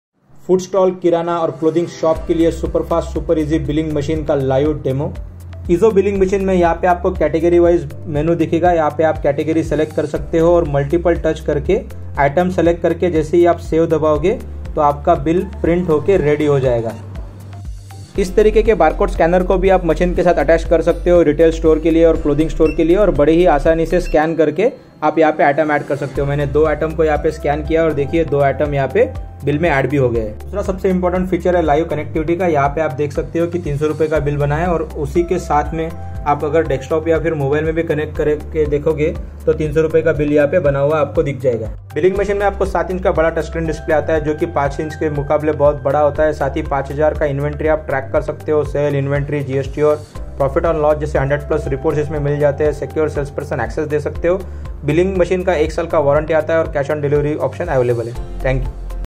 Billing Machine for Fastfood, Kirana - Live Demo
Best Billing Machine for Hotel, Restaurants, Fast Food, Retail Shop, Super Market, Grocery Shop
7" Touch Screen Display
Connect with Mobile or Desktop
5000+ Inventory Live Tracking
Get 100+ Business Reports